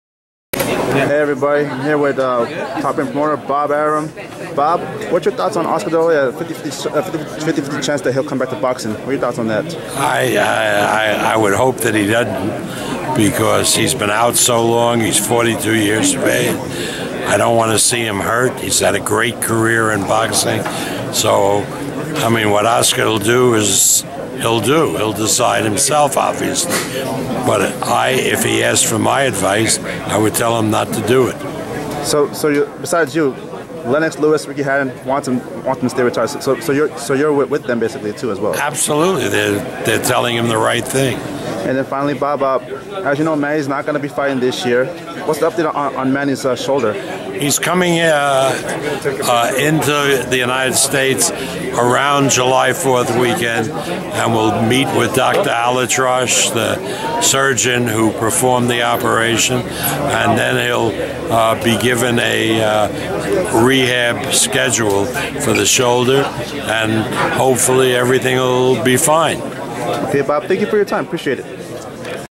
Bob Arum-"I don't wanna see Oscar De La Hoya hurt (if he comes back to boxing)" & Pacquiao update
Spoke to Bob Arum and asked him what is his reaction to Oscar De La Hoya with a 50/50 possiblity of coming back to boxing and gives an update on Manny Pacquiao since the right shoulder surgery. Bob told me that Manny will be in the United States next month.